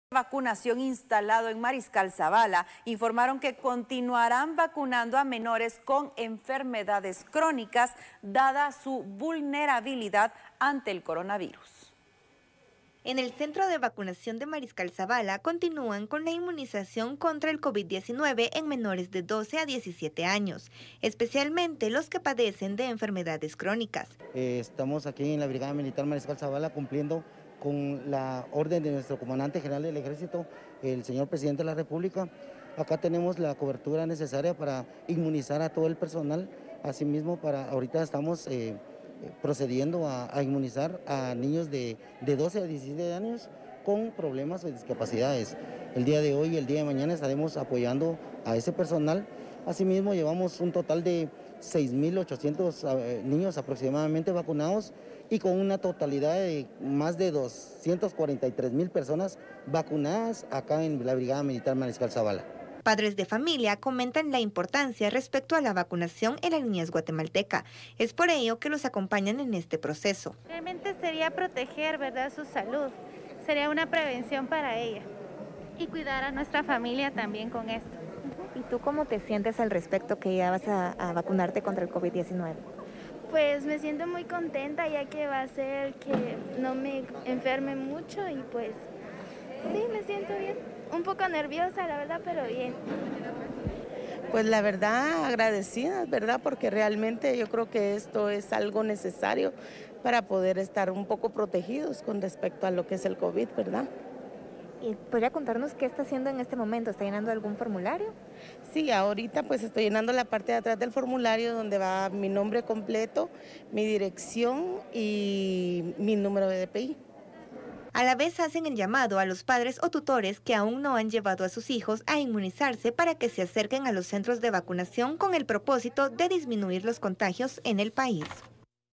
Más de 6 mil 800 niños inmunizados en Mariscal Zavala
Instan a la población a participar en el proceso

En el centro de vacunación de Mariscal Zavala se están vacunando a menores de edad desde los 12 años en adelante en especial aquellos con discapacidad. Visita nuestro sitio Visita nuestro sitio